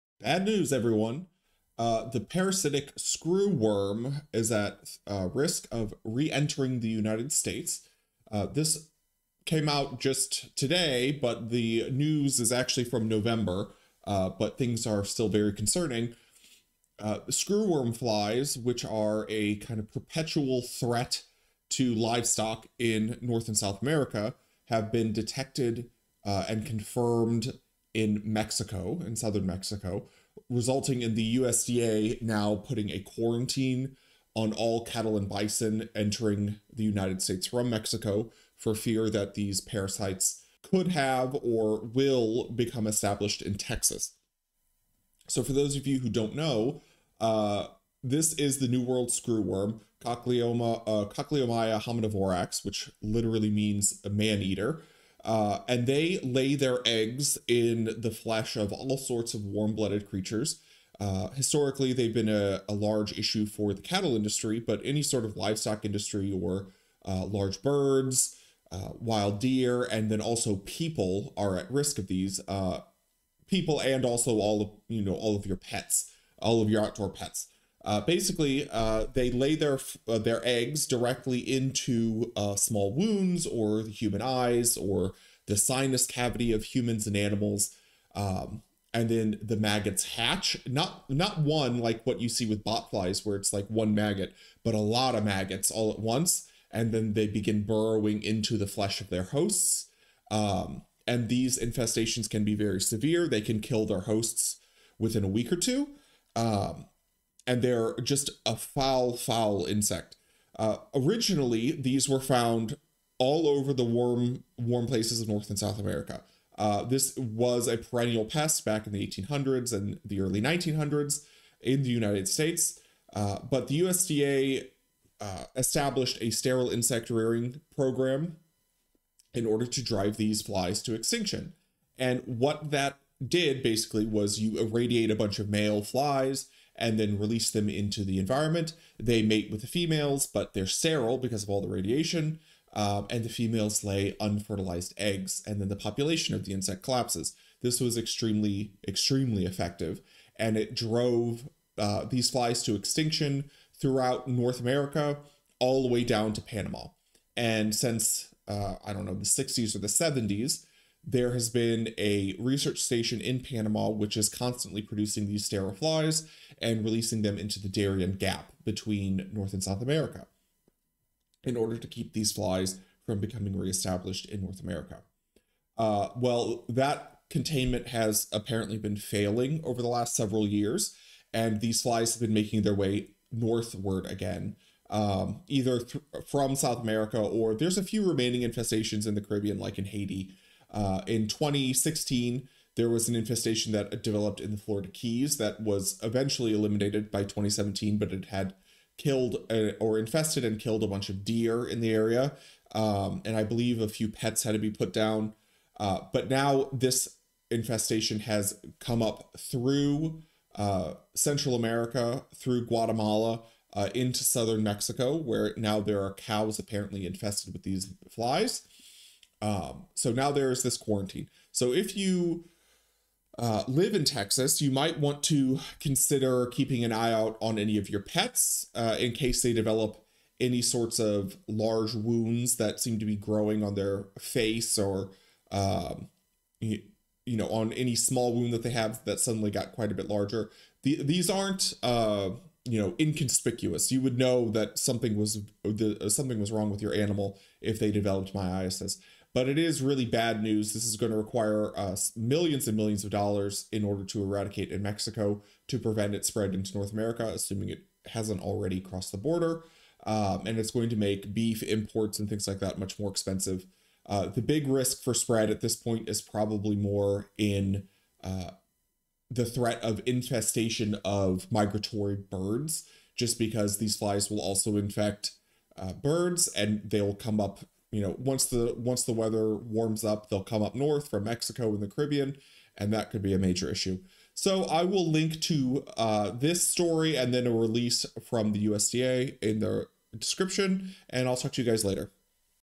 Deadly Parasite of Humans and Animals Found in Mexico
Deadly screwworm flies have been migrating north from South America and now are threatening invasion of Texas